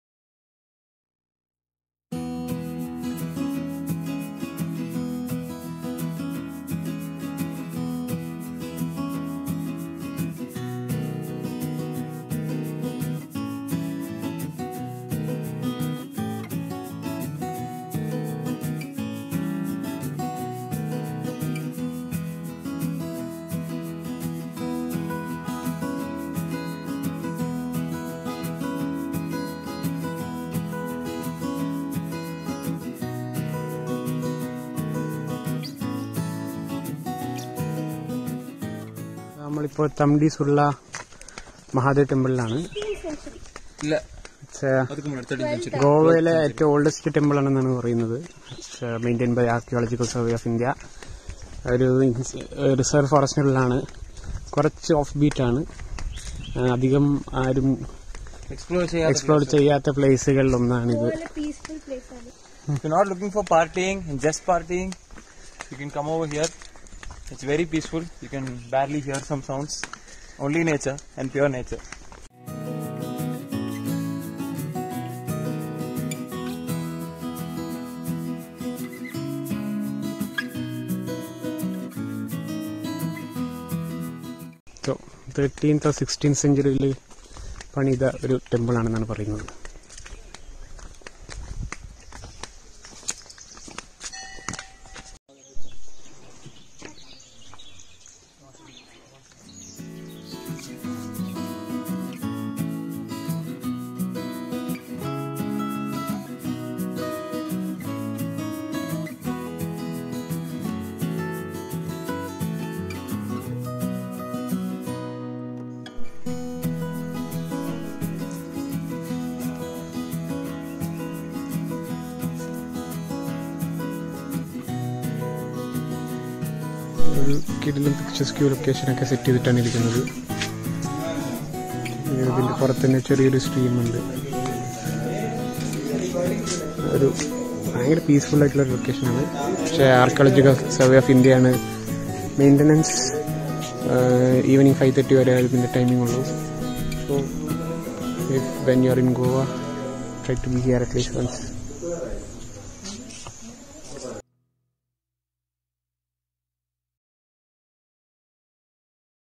Diaries of thewanderlusttechie | S2 | Goan Days | E2 | "The Hidden Gem"
This episode of the Goan Dyas series takes you to a less explored place in Goa, A 13th-century Mahadev temple in Molem wildlife sanctuary. This sure is a must-visit for peace lovers. Spend an evening amidst nature with all the kinds of birds chirping around you.